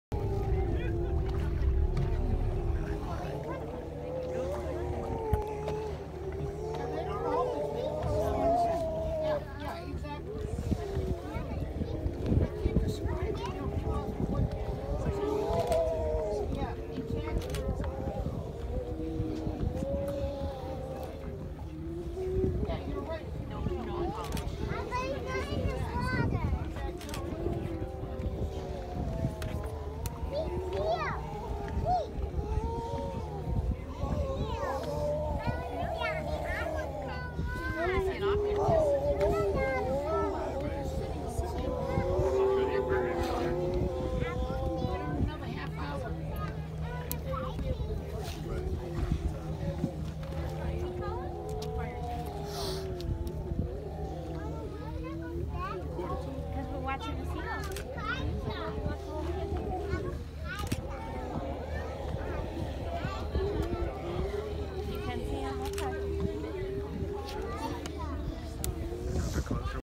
Grey Seals at Cape Cod, MA 8/14/18
We went seal watching with Monomoy Island Excursions in Cape Cod, MA on 8/14/18. These are Grey Seals vocalizing.